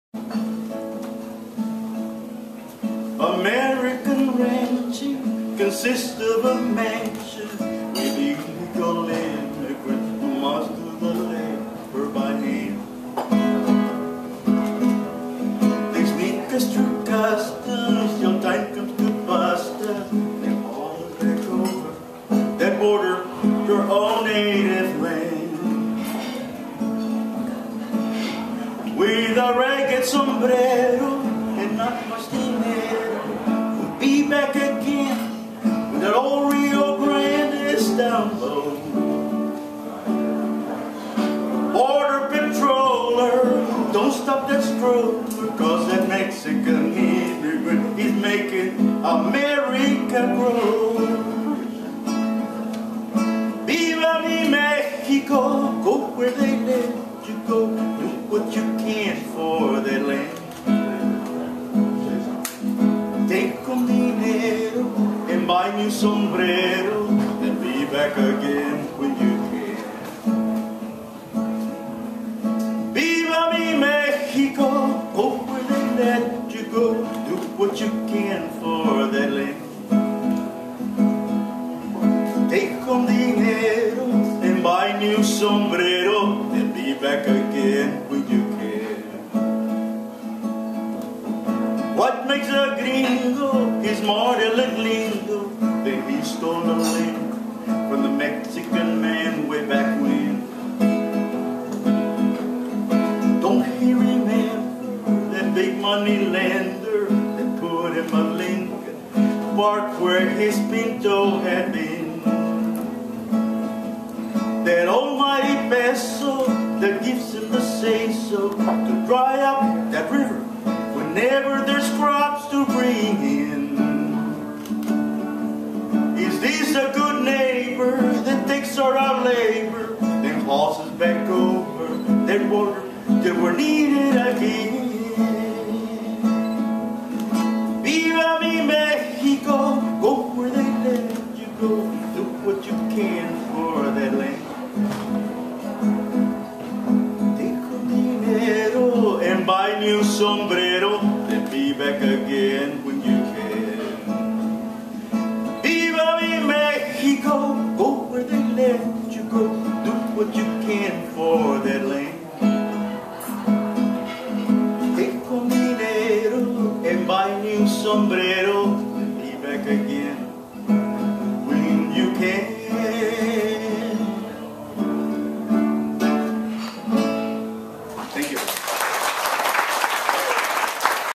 Rudy Arredondo performs at Food Justice: A Global Issue
The Accokeek Foundation held the first event in its four-part Food Justice Series on Thursday, March 31, 2011. In celebration of César Chávez Day, the event explored the struggles that are faced by farmworkers around the world and the significant role that small family farmers play in the fight for sustainability and social justice.